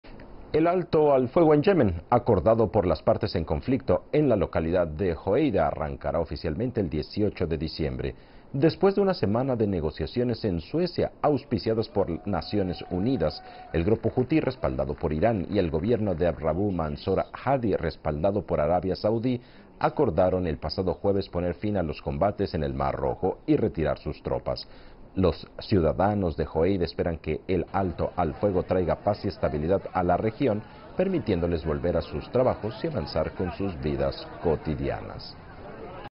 El alto al fuego en Yemen comienza oficialmente el 18 de diciembre
El alto al fuego en Yemen acordado por las partes en conflicto en la localidad de Hoeida arrancará oficialmente el 18 de diciembre. Después de una semana de negociaciones en Suecia auspiciadas por las Naciones Unidas, el grupo hutí respaldado por Irán y el gobierno de Abd-Rabbu Mansour Hadi respaldado por Arabia Saudí acordaron el pasado jueves poner fin a los combates en el Mar Rojo y retirar sus tropas. Los ciudadanos de Hoeida esperan que el alto al fuego traiga paz y estabilidad a la región, permitiéndoles volver a sus trabajos y avanzar con sus vidas cotidianas.